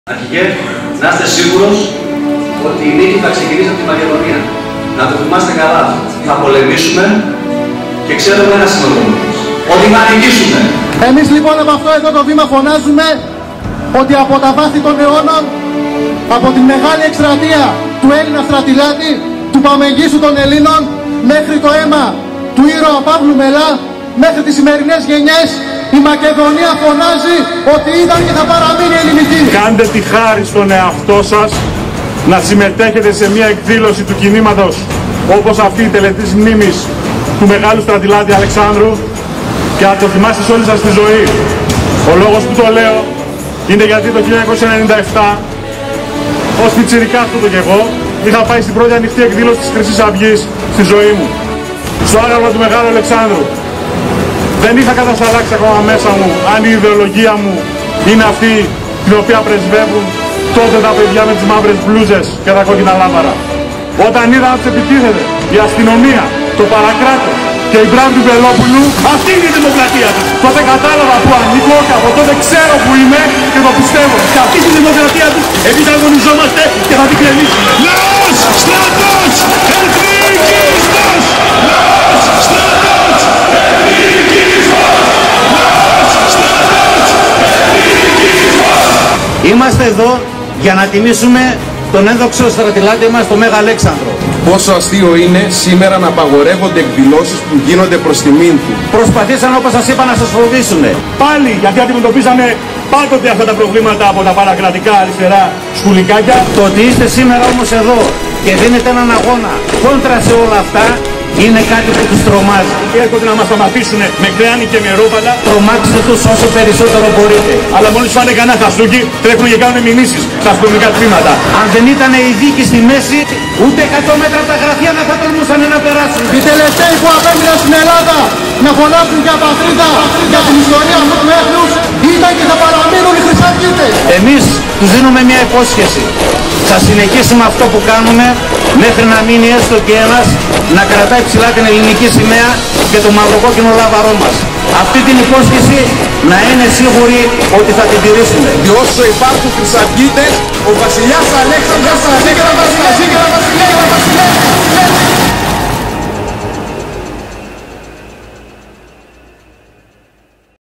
«Τα παιδιά με τις Μαύρες μπλούζες» Μακεδονία → Χρυσή Αυγή → Μέγας Αλέξανδρος
Τα παιδιά με τις Μάυρες μπλούζες. Ο Αρτέμης Ματθαιόπουλος, ο Χρήστος Χατζησάββας, ο Γιάνης Λαγός, ο Φώτης Γραικός & ο Γιάννης Σαχινίδης. Θεσσαλονίκη 25/06/2017